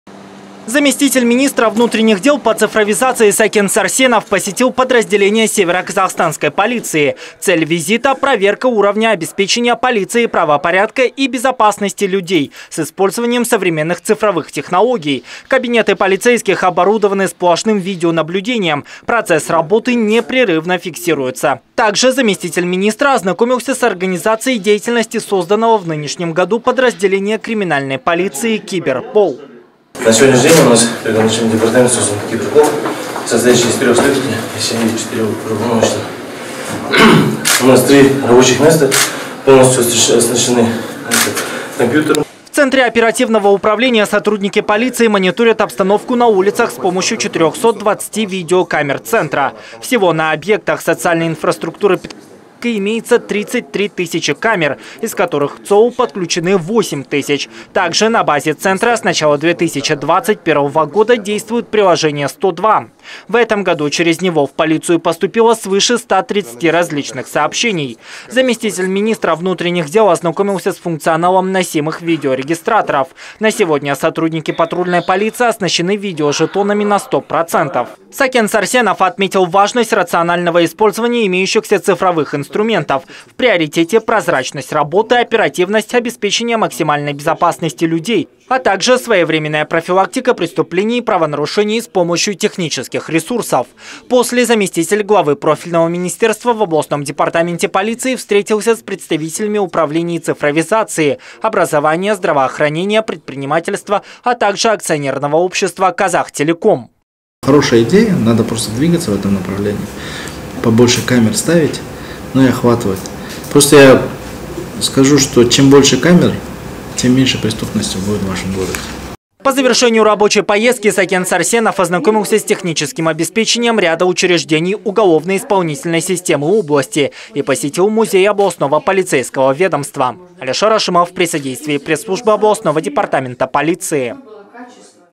Заместитель министра внутренних дел РК посетил СКО
Заместитель министра внутренних дел по цифровизации Сакен Сарсенов посетил подразделения североказахстанской полиции. Цель визита - проверка уровня обеспечения полицией правопорядка и безопасности людей, с использованием современных цифровых технологий. Кабинеты полицейских оборудованы сплошным видеонаблюдением, процесс работы непрерывно фиксируется. Также заместитель министра ознакомился с организацией деятельности созданного в нынешнем году подразделения криминальной полиции - «Киберпол».